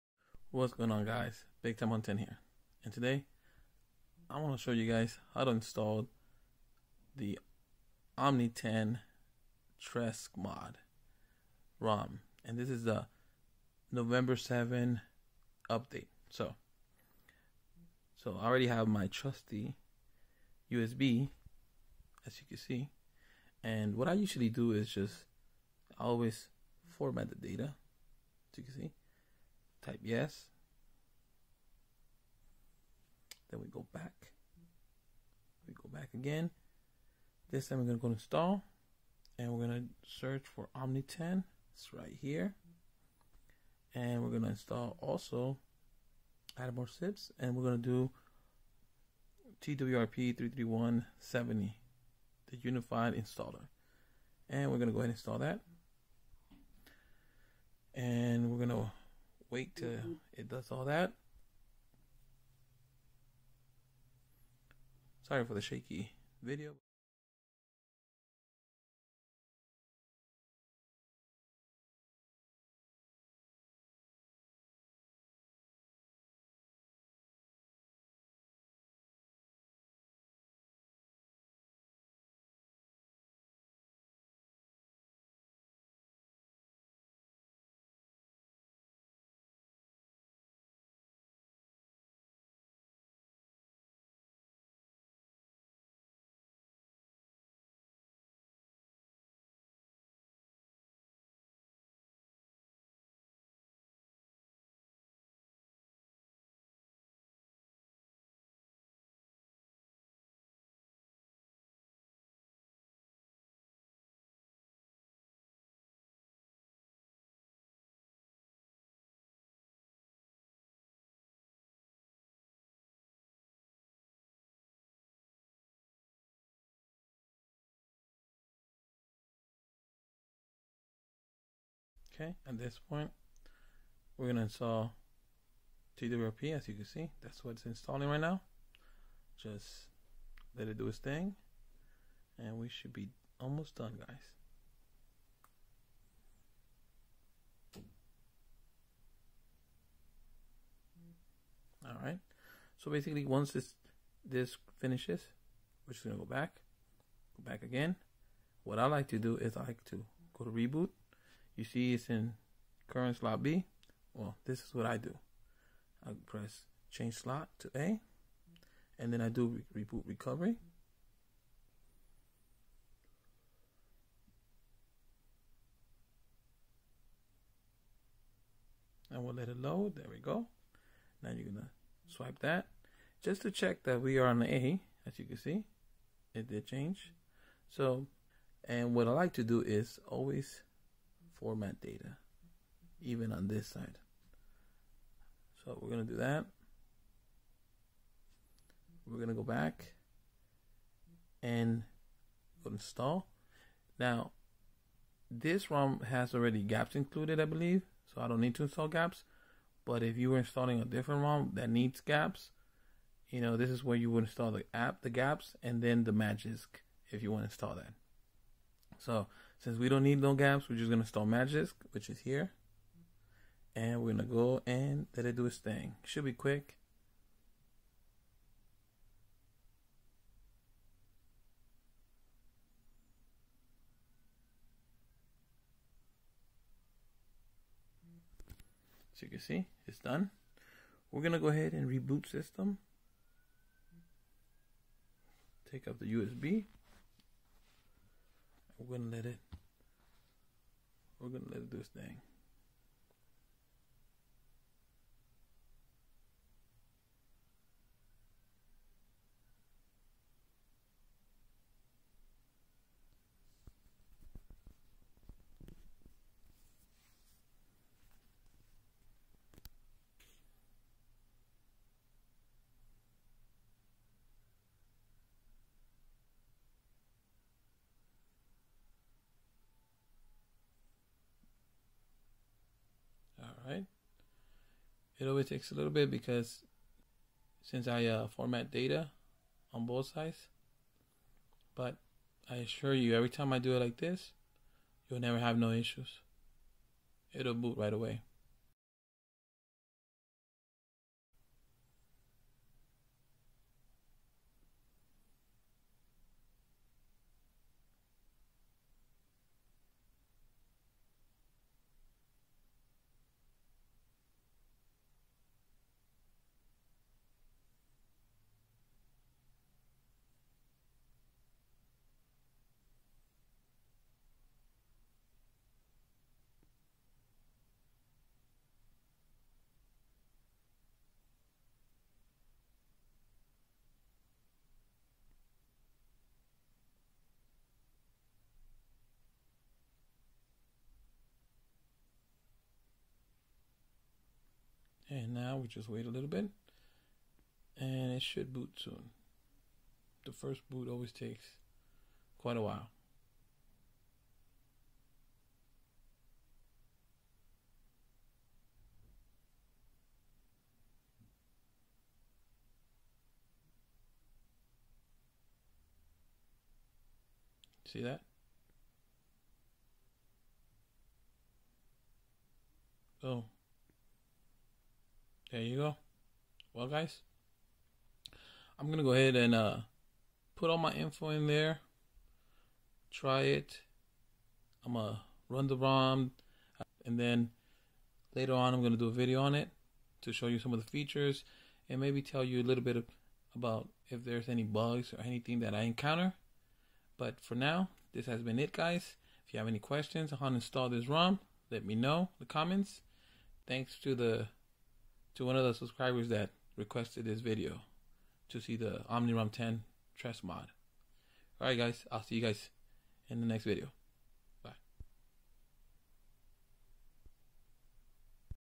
Omni 10 Treskmod Install [One plus 7 Pro]
Just a quick video how to install a Custom Rom.